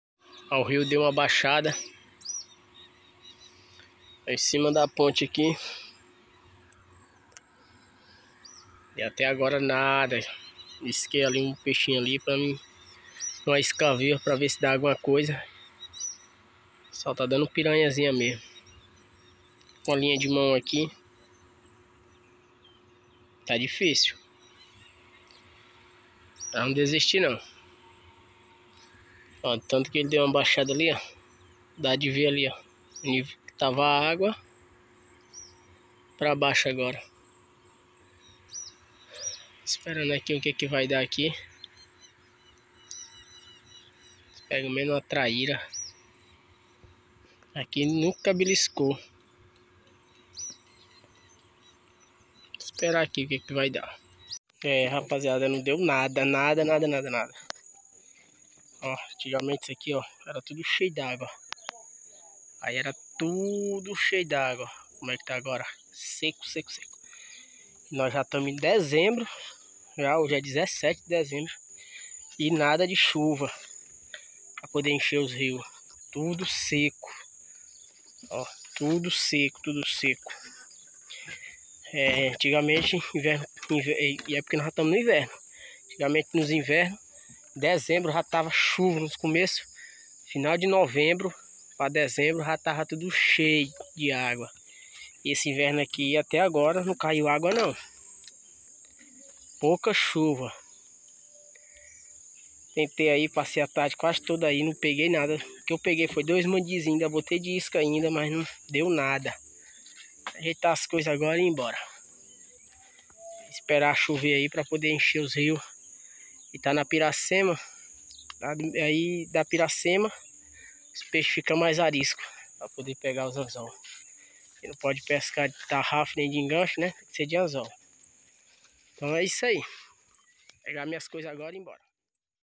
o inverno tá fraco no Maranhão
fala glr o enveno tá muito fraca no Maranhão não tá chovendo